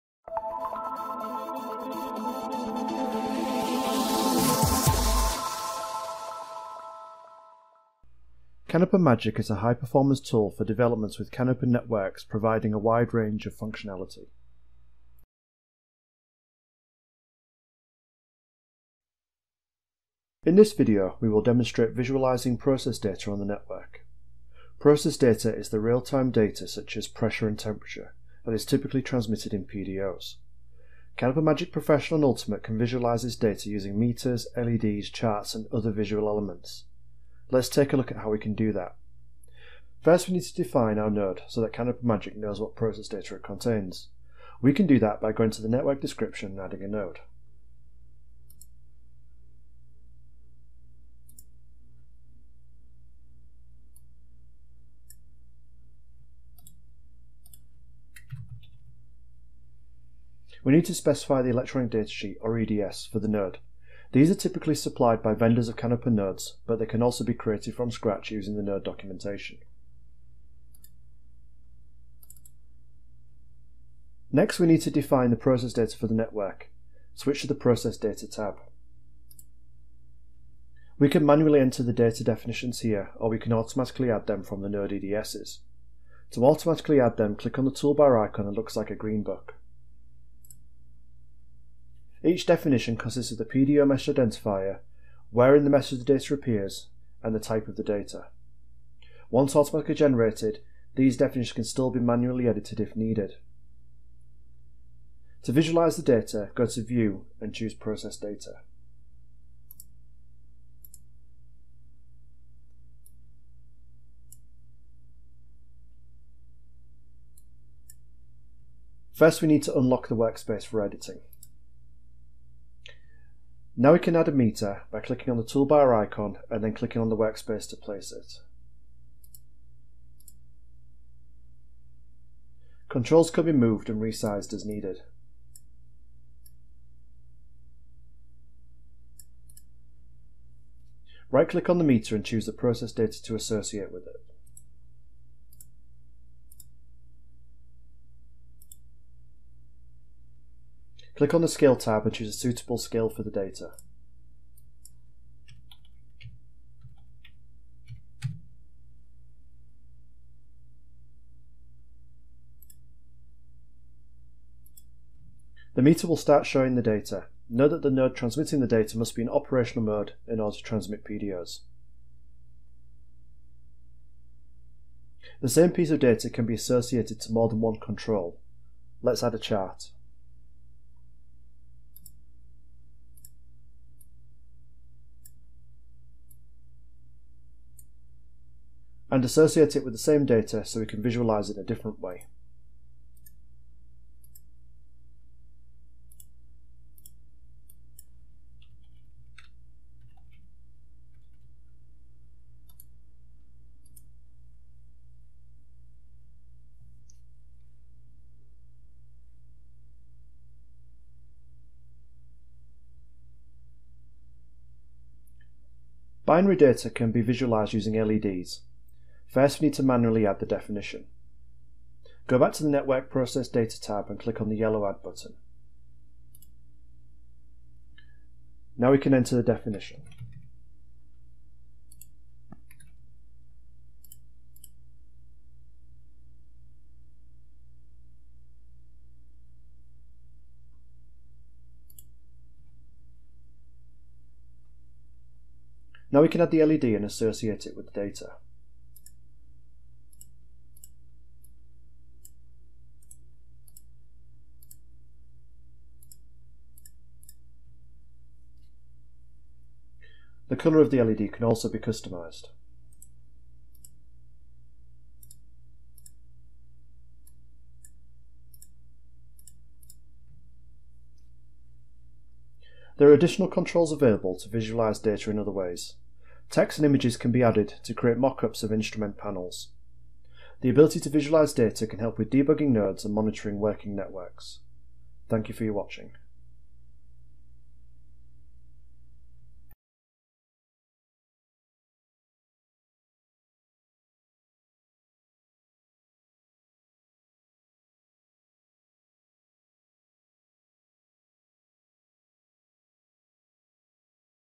CANopen Magic - Visualize Data
A demonstration of how to visualize real time data using meters, charts and LEDs on a CANopen network.